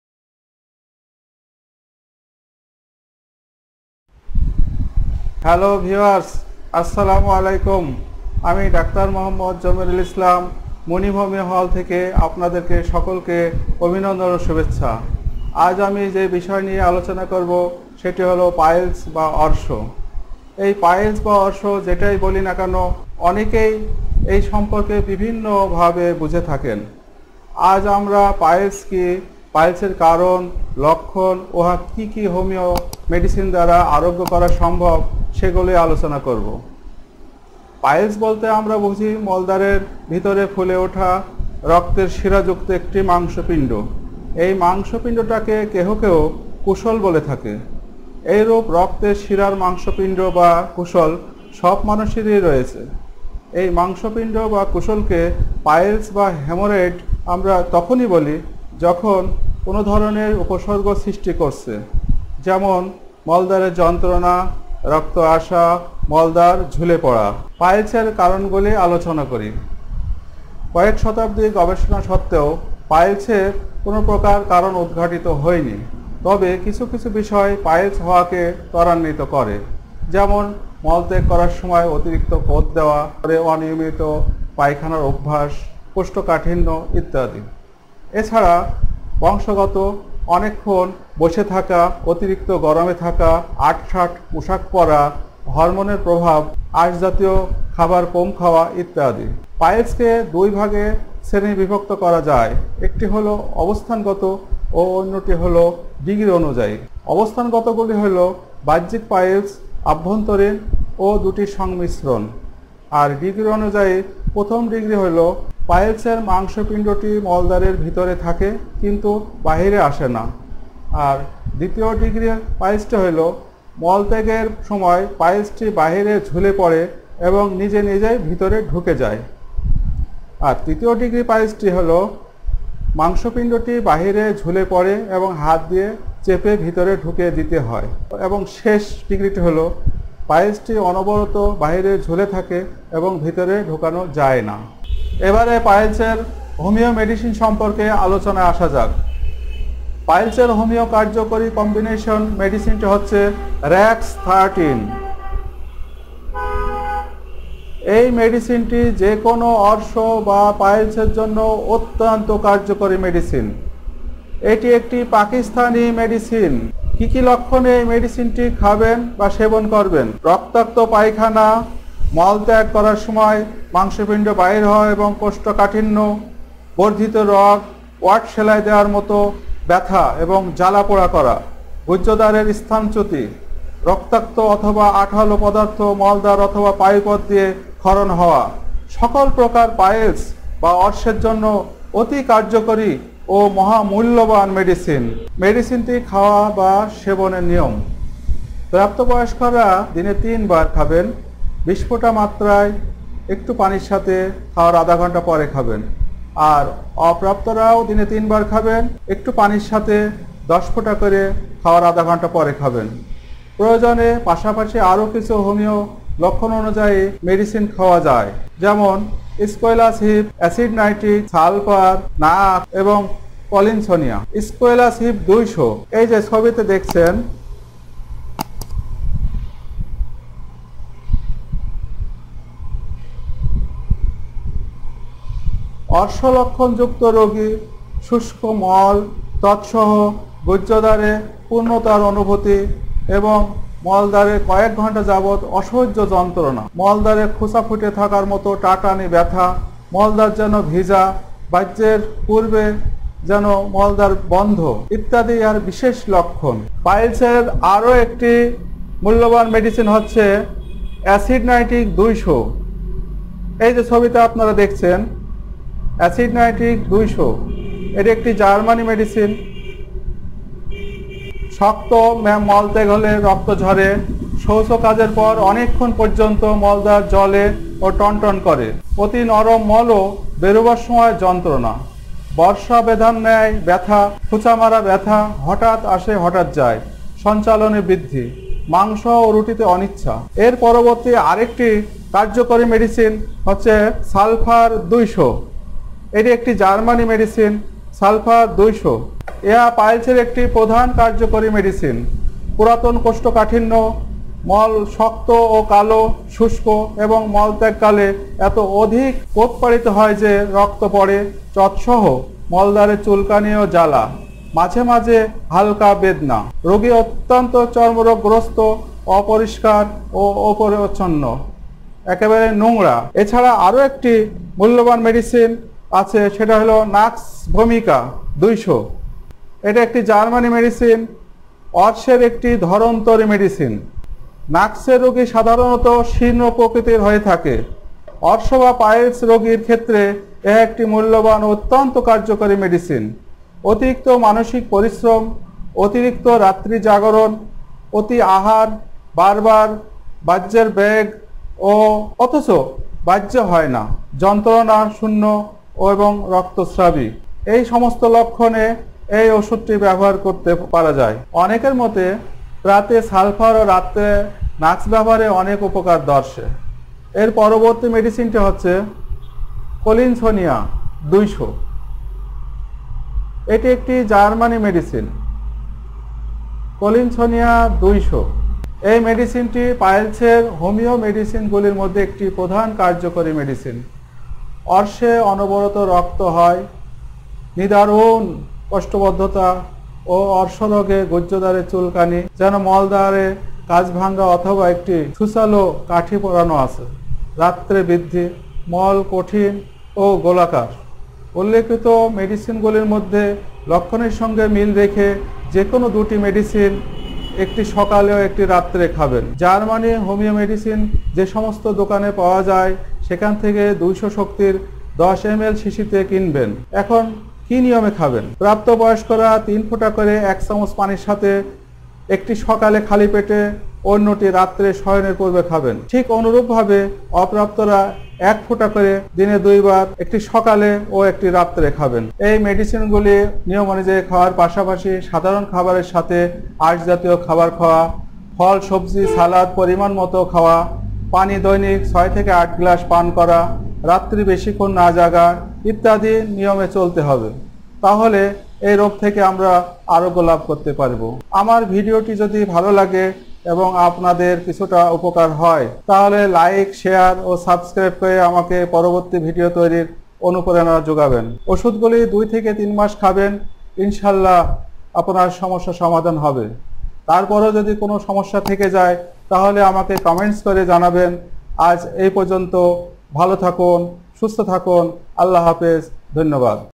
পাইলস থেকে মুক্তি পেতে আমাদের ভিডিওটি মনোযোগ দিয়ে দেখুন । সঠিক সময়ে চিকিৎসা করালে পাইলস ভালো হয় ।
পাইলস এর জন্য নিচের হোমিও ওষুধ গুলো খুবই কার্যকর
1. RAX 13
2. Aesculus 200
3. Acidum Nitricum 200
4. Salfer 200
5. nux Vumica 200
6. Collinsonia canadensis 200

ওষুধ গুলোর সম্পূর্ণ ব্যবহার বিধি ভিডিওতে দেওয়া আছে ।

Dr. Md. Jomirul Islam